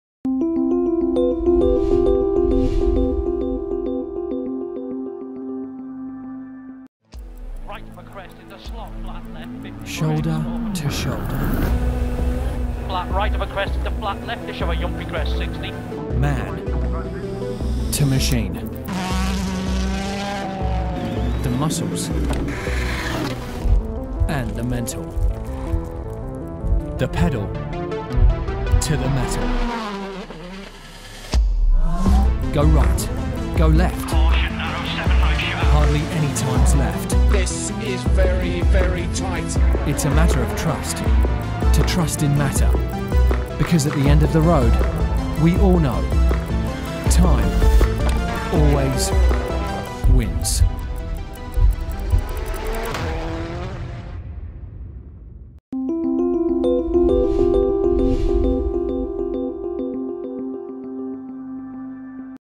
TW Steel x WRC l Jura Watches
TW Steel team up with FIA World Rally Championship as the official watch partner. Shop TW Steel today at Jura Watches!